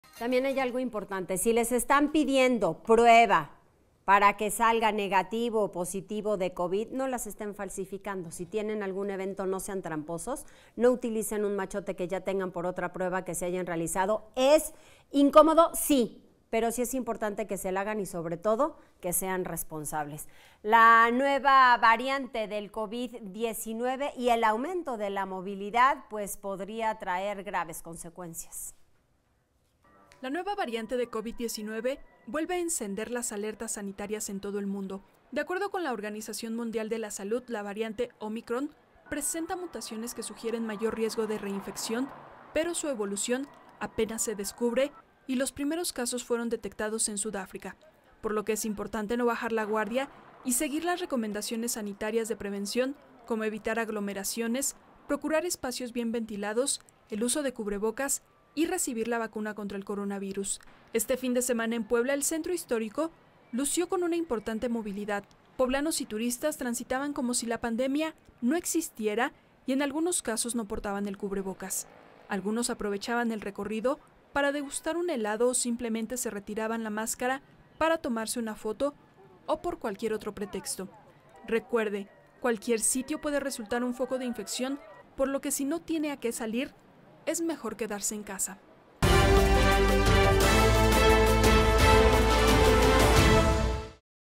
Alerta mundial | Las Noticias Puebla
Es importante seguir con las medidas sanitarias, la nueva variable del covid-19 podría traer graves consecuencias.